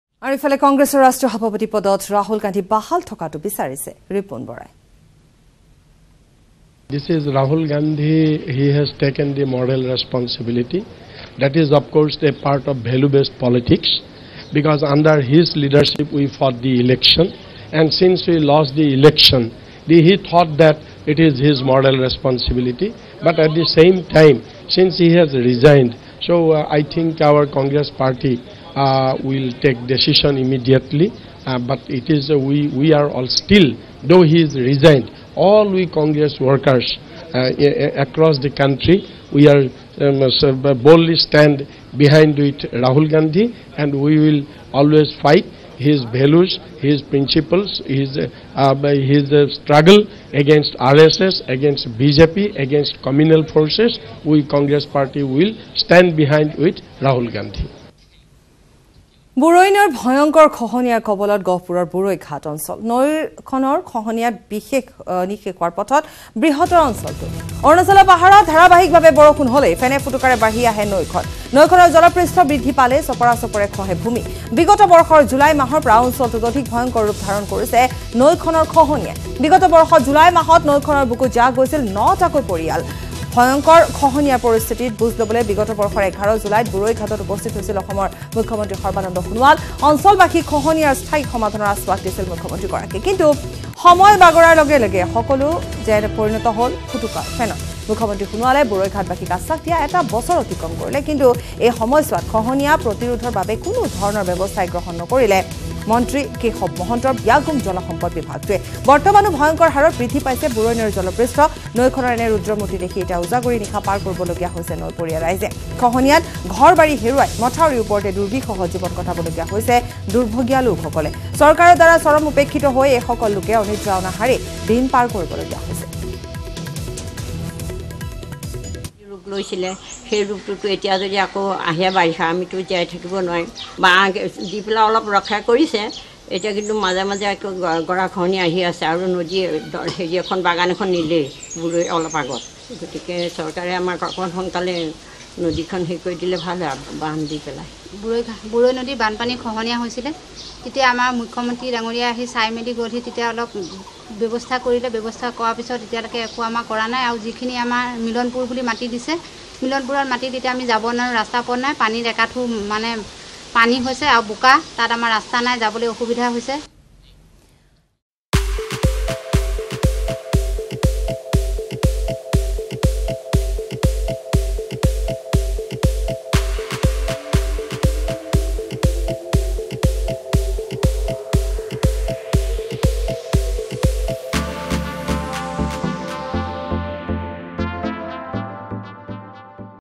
"All Congress Workers Boldly Stand With Rahul Gandhi And Will Work With His Values" Says Ripun Bora
Ripun Bora has shown his support